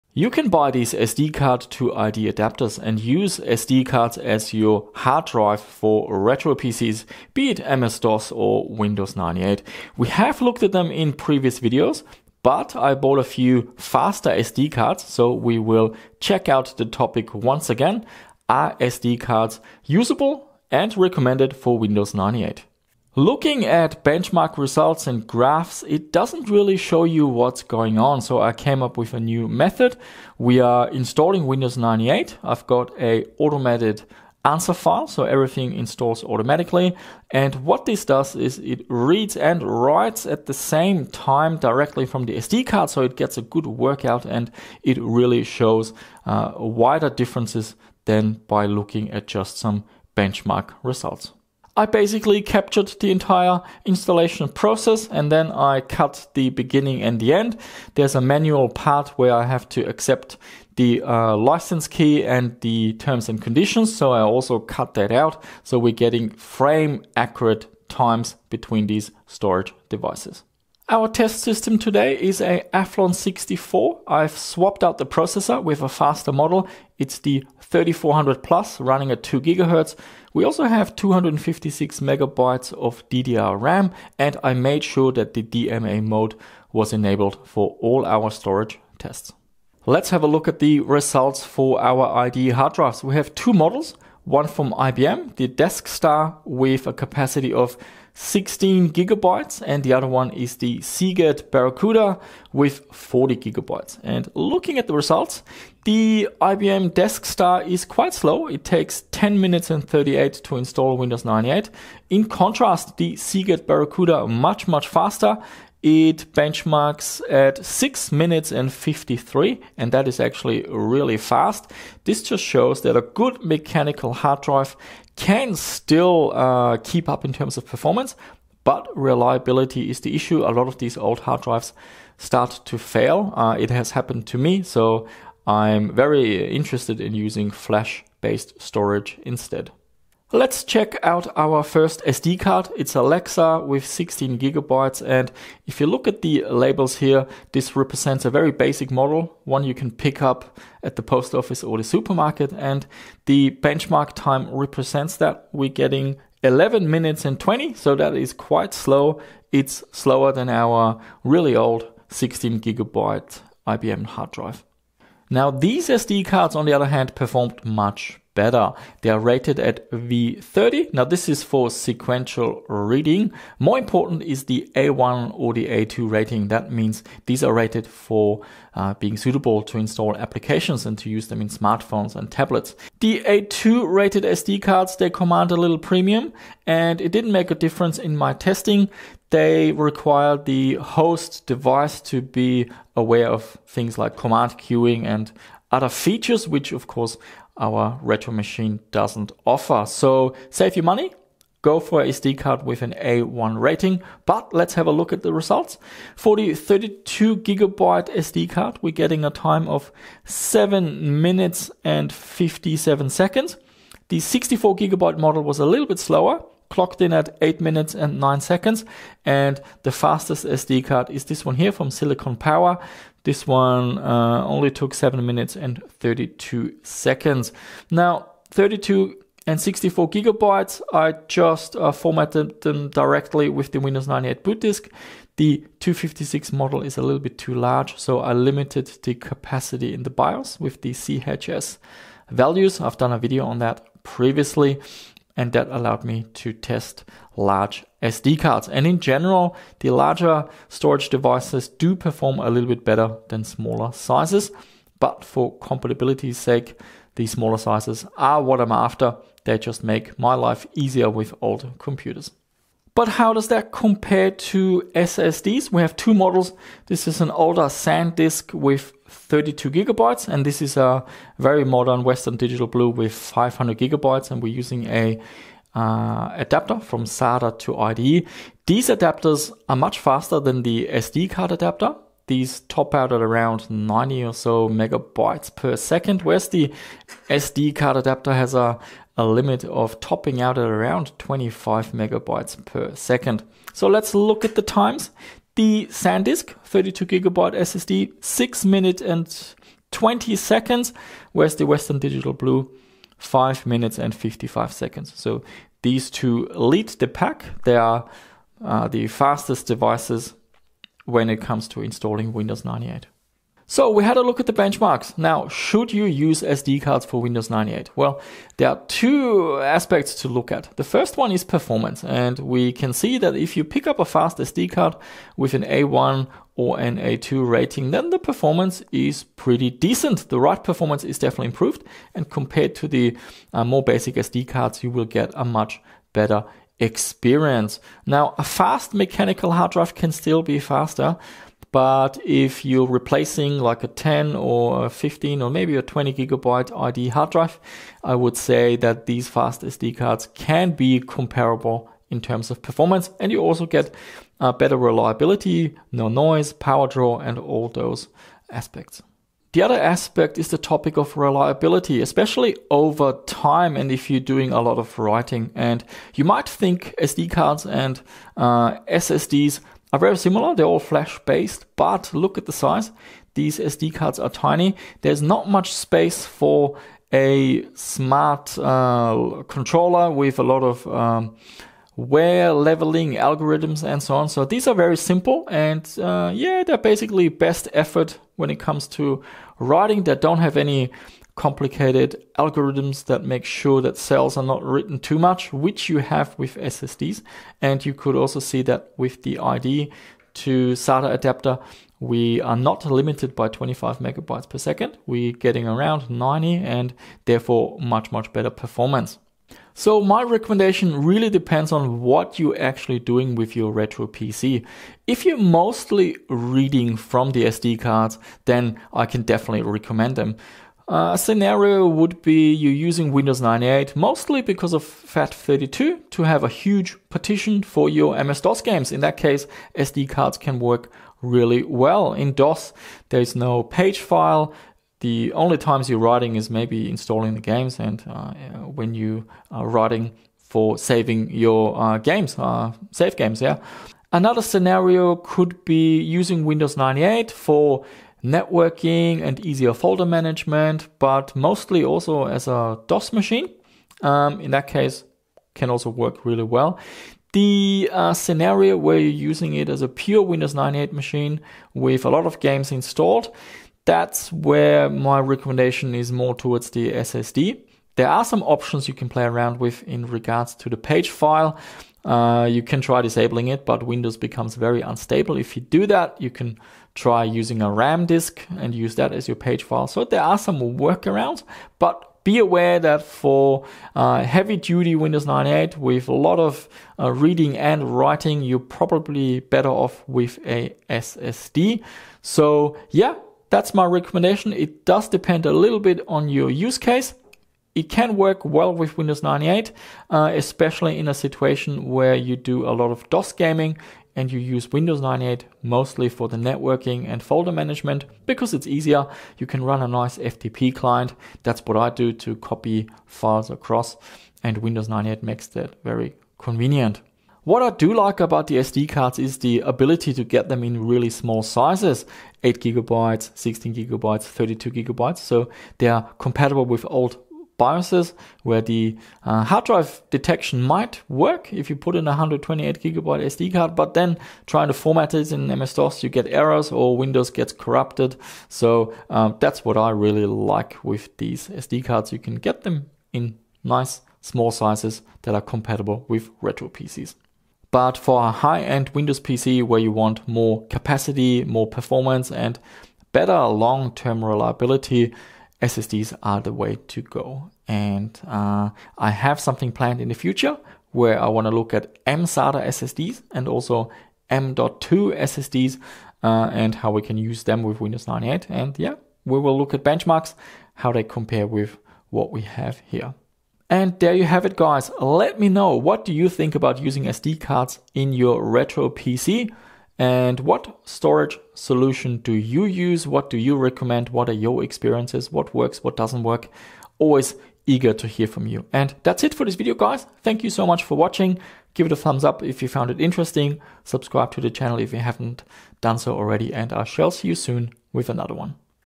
Run Windows 98 on SD Card vs HDD and SSD
SD cards have improved with their performance I bought newer faster models and we are testing how they perform with Windows 98 instead of using traditional Hard Drives or Solid State Drives. Make sure you use SD cards with A1 or A2 rating as they have better write performance.